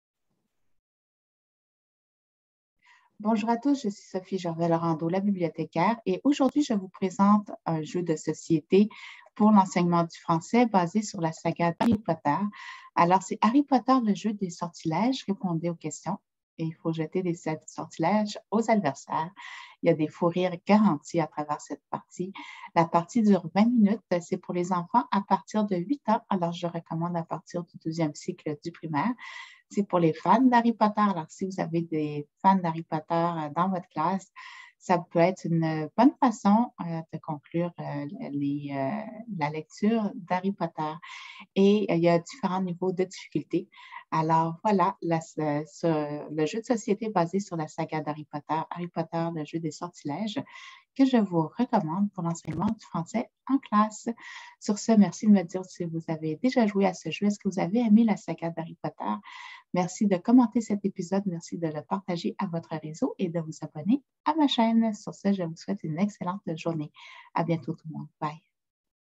Le 13 novembre 2021 Jeu de société Harry Potter pour l'enseignement en classe
Sophie Gervais-Laurendeau, La Bibliothécaire, Le 13 novembre 2021, vous présente le jeu Harry Potter - le jeu des sortilèges, basé sur la Sage. Je le recommande pour l'enseignement à l'école. Avez-vous déjà joué à ce jeu? Avez-vous aimé la saga d'Harry Potter? Bonne écoute!